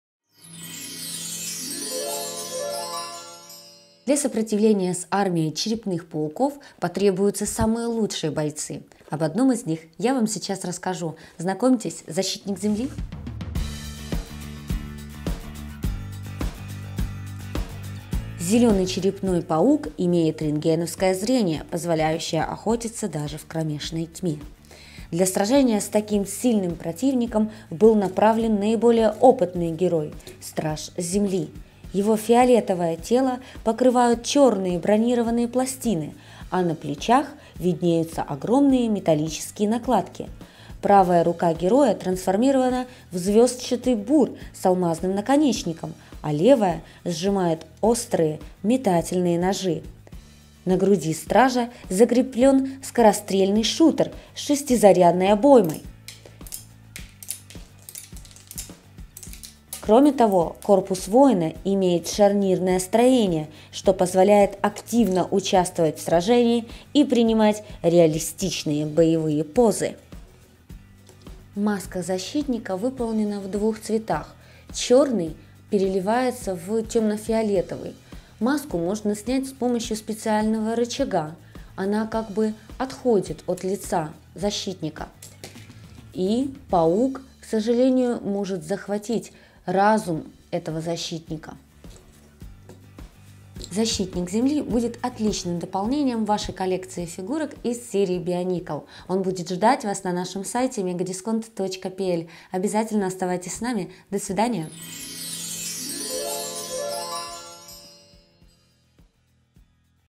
Protector of Earth / Страж Земли - Lego Bionicle / Lego Биониклы - 70781
Подземная тревога! Паучий Череп атакует кристаллическую шахту. Вращай мощный кристаллический звездный бур земли Стража, чтобы пробурить скалу и дать отпор злобному захватчику. Если Паучий Череп дотянется до головы Стража BIONICLE, он может сдвинуть переключатель и сорвать маску. Освободи стихийную мощь скорострельного шутера, чтобы держать врага на расстоянии и сохранять контроль под землей.

Defend the underground mine with the Protector of Earth! Defeat the Skull Spider with the LEGO BIONICLE Protector of Earth with mask pop-off trigger, spinning Star Drill and rapid shooter. Underground alert! The crystal mine is under attack from the Skull Spider. Spin the Protector of Earth's powerful crystal Star Drill to bore through rock and confront the evil intruder. If the Skull Spider latches onto the Protector's BIONICLE head, it can pop the trigger and release the mask. Unleash the Elemental power of the rapid shooter to fend off the villain and keep control of the underground. Features a BIONICLE head with dual-tone mask and mask pop-off trigger, BIONICLE shell, rocky shoulders, spinning Star Drill, Elemental chest armor with rapid shooter and ammunition, and posable joints. Also includes a pale green Skull Spider.